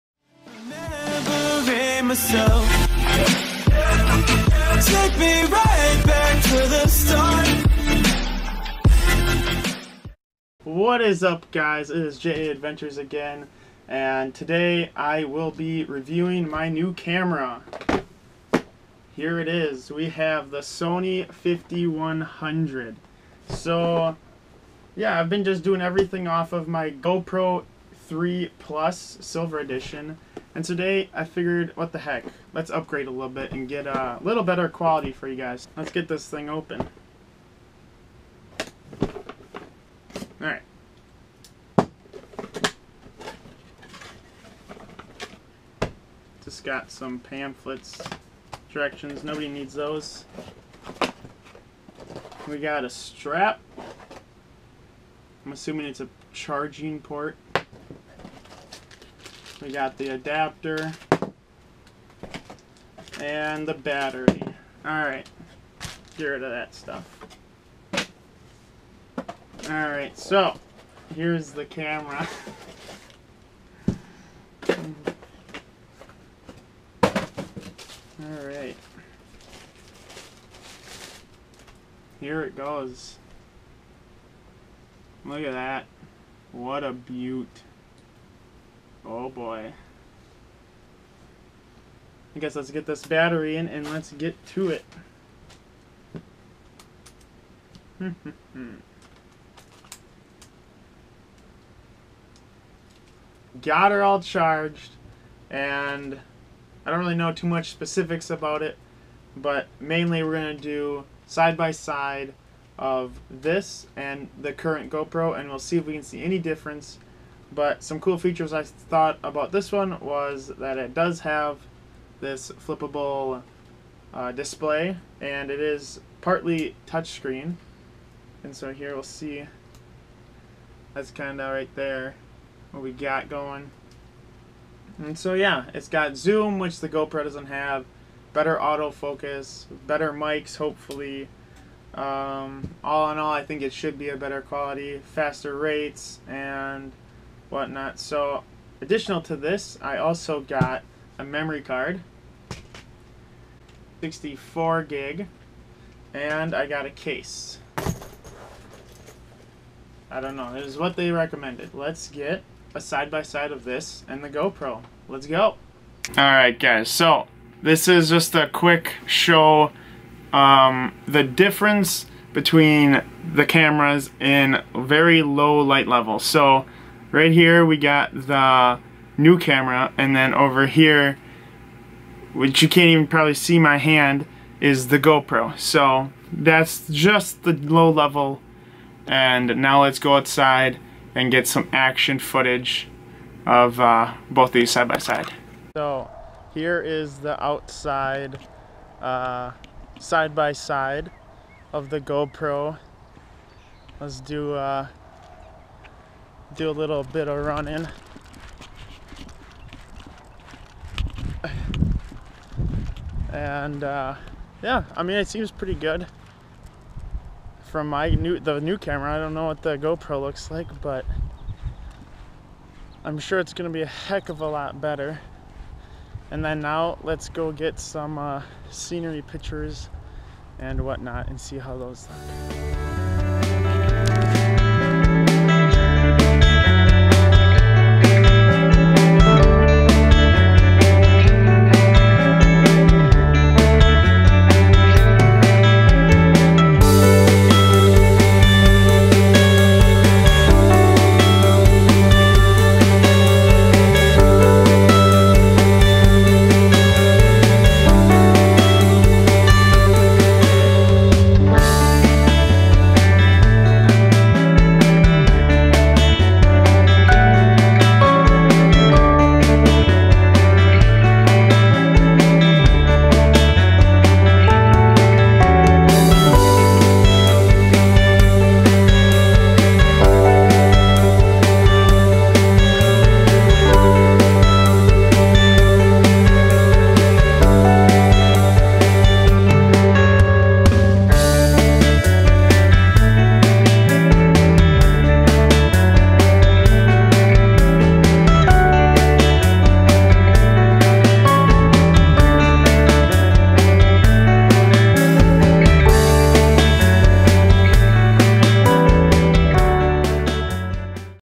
Sony 5100 VS GoPro 3+ Silver
We are compairing my new sony 5100 camera against my Gopro hero 3 + silver. I wonder what one will be better. Watch to find out